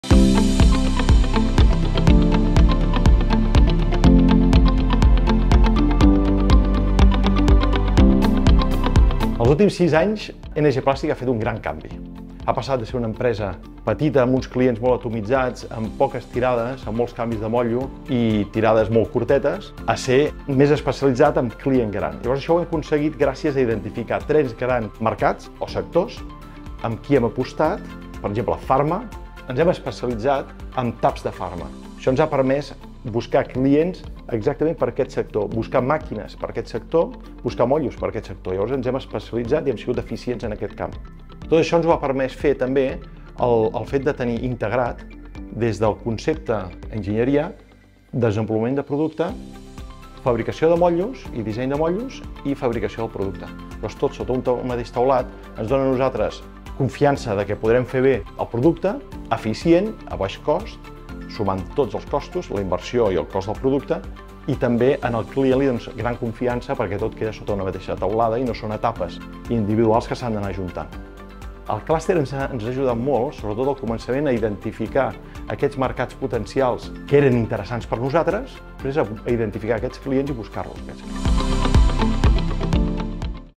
NG Plastics guanya el reconeixement al Canvi Estratègic i el Creixement
Als 10 anys del Packaging Cluster (1 de desembre 2022) es van lliurar uns reconeixements a diverses empreses del clúster, on en aquest cas, va destacar NG Plastics pel seu Canvi Estratègic i Creixement.

Toni Borrell, CEO de NG Plastics, va recollir el reconeixement.